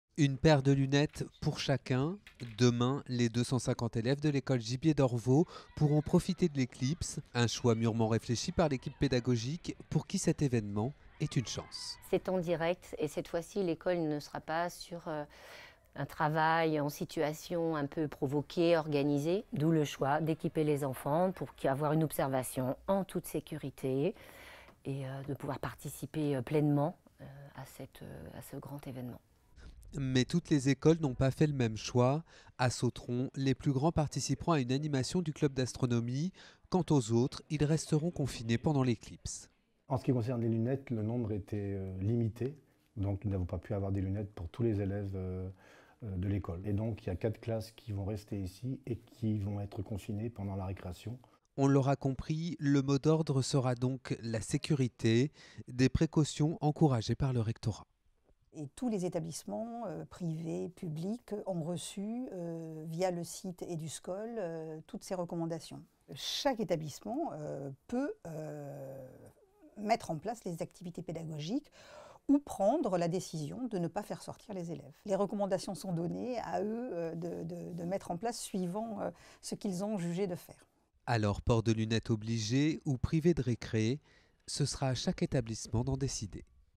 Attention éclipse ! Les écoles se préparent
Les écoles de Loire-Atlantique ont déjà tout prévu pour demain matin. Port de lunettes obligatoire ou suppression de la récré, chaque établissement avait le choix. Exemples avec les écoles élémentaires Gibier à Orvault et La Rivière à Sautron.